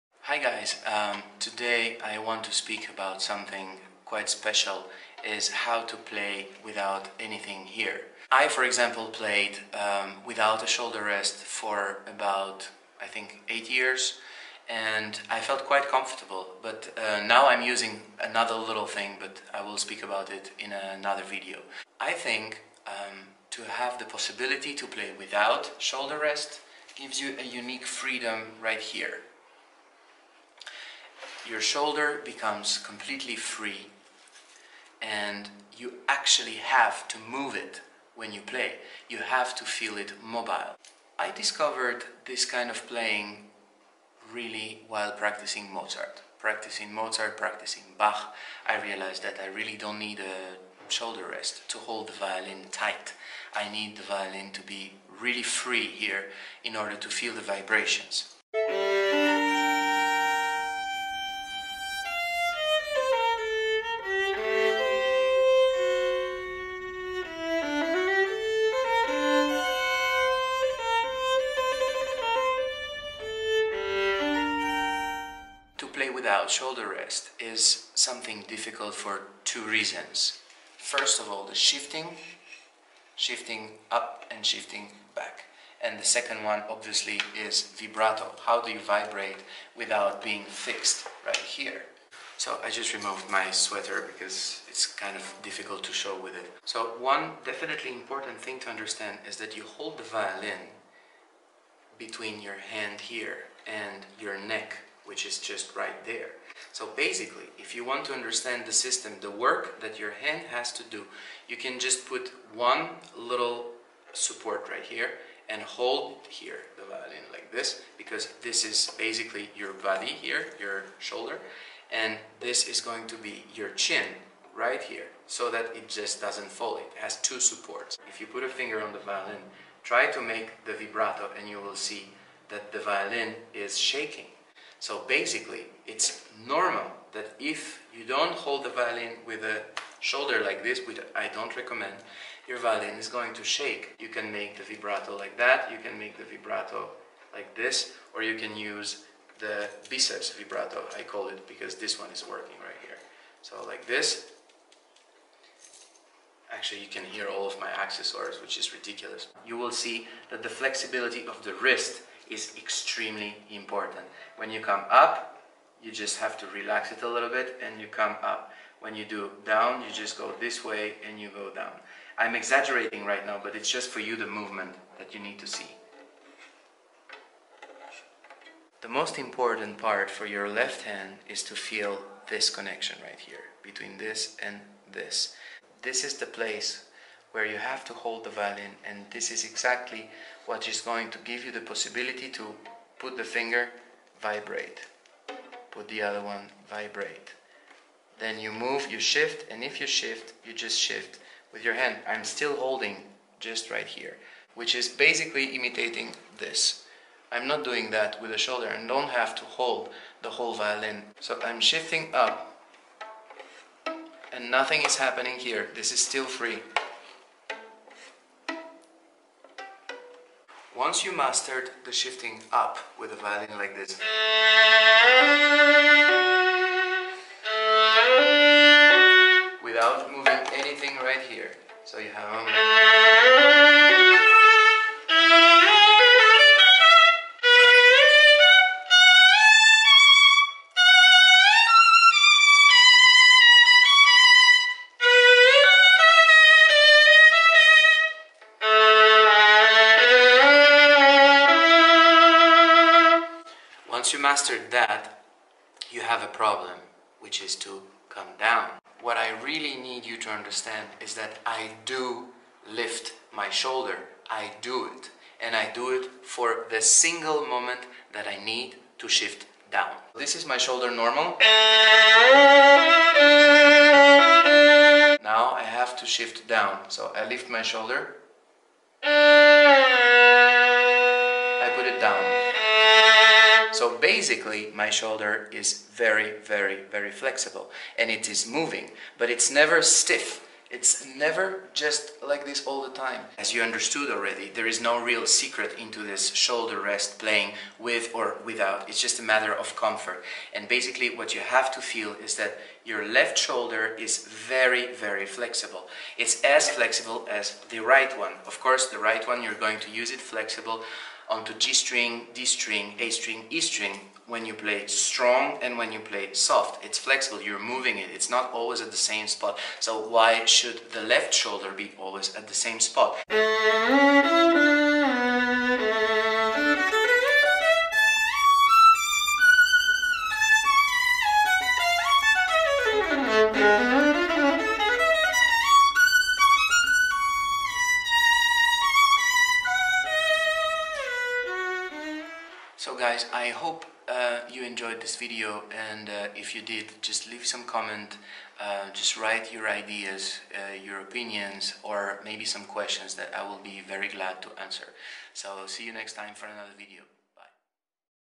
Playing without Shoulder rest! Yes Or No?
In this video I want to show my way of playing without Shoulder rest, and make clear that it is a really different approach!
The goal is to be able to feel comfortable and extremely free.
For each people the process is different, this one is mine, and I am glad to share it if it can help.
Shifting and vibrato are the recurrent issues of playing without shoulder rest, so you will have my little insight on the question)